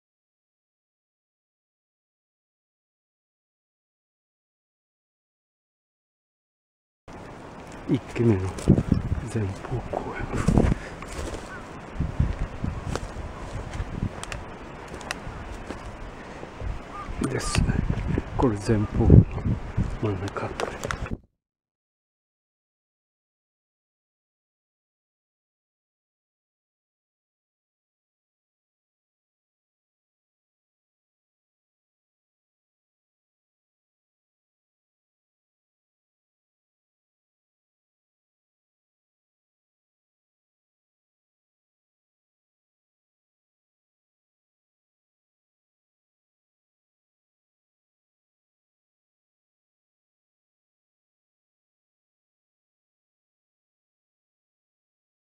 北大塚＆姫塚２（岩清尾山）（前期）（香川県）Kitaootsuka&Himezuka２ Tumulus(Kagawa Pref.)
今回紹介する前期古墳は香川県高松市の岩清尾山古墳群の北大塚古墳と姫塚古墳です。一墳丘全体がごつごつした石に覆われています。積石塚です。詳しくはライブドアブログ
古墳を動画で見るサイトをご覧ください。以下がURLです。
ご覧になる方法が参考になります。